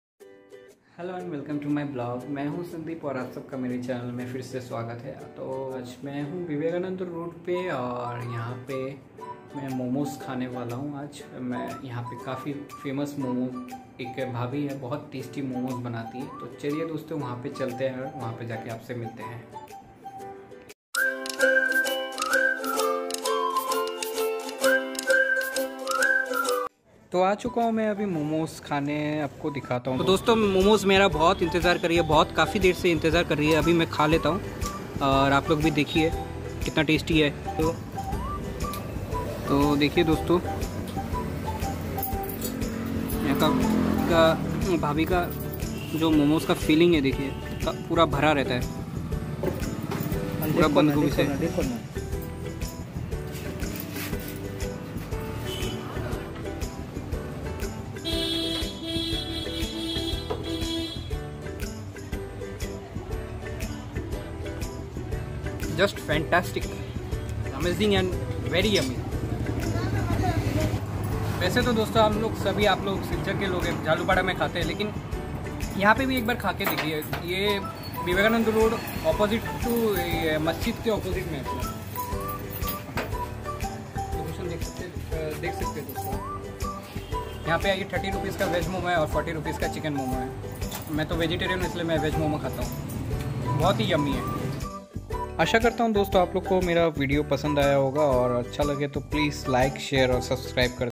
Vivekananda Road ka super tasty momos|| Silchar || Assam
Address- Vivekananda Road, opposite Mosque